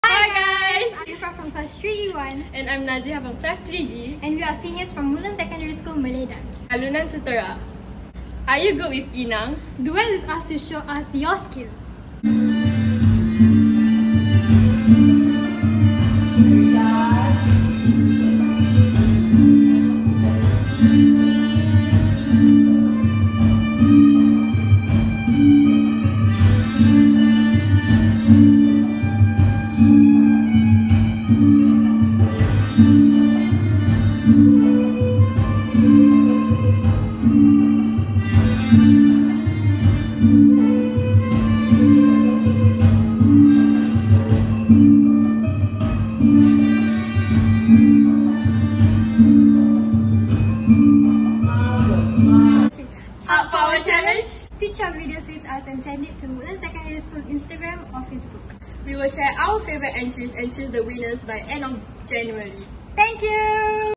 WDL CCA Challenge - Malay Dance
Our CCA Seniors have issued a challenge! Ready to show us your skills? Duet their video and send us your performance at our Facebook or Instagram. We will choose our favourite entries by end January.

Not sure your gifts are?